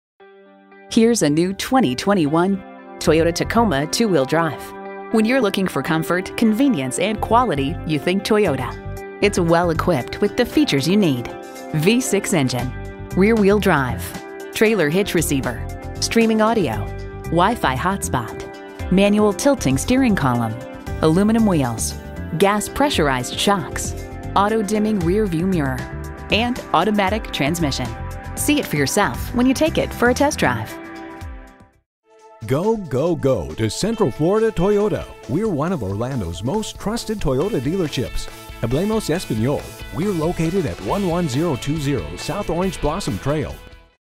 New 2021 Toyota Tacoma 2WD Orlando, FL
Year: 2021
Make: Toyota
Model: Tacoma 2WD
Trim: SR5 Double Cab 5' Bed V6 Automatic
Engine: 3.5L V6 CYLINDER
Transmission: 6-Speed Automatic
Color: Silver Sky Metallic
Interior: Cement
Stock #: MM151036
VIN: 3TMAZ5CN4MM151036

Address:
11020 S Orange Blossom Trail